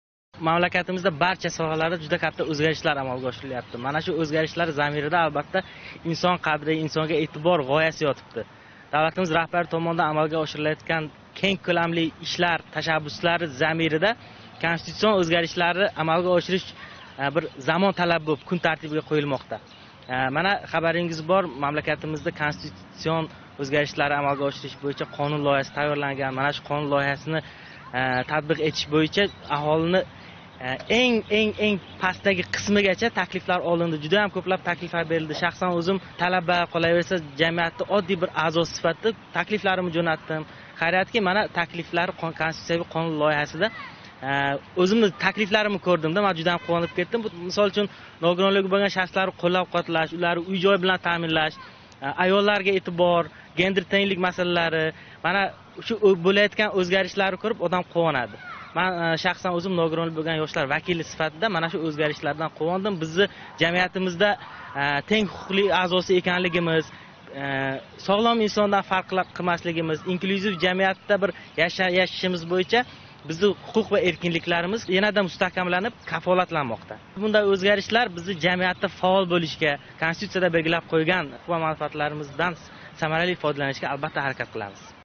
Namangan MTRK | Yangilanayotgan Konstitutsiya — huquq va erkinliklarimiz yanada mustahkamlanmoqda
Javlon Abdullayev,
xalq deputatlari Kosonsoy tuman Kengashidagi O‘zXDP guruhi a’zosi,
"Kelajak liderlari" klubi a’zosi.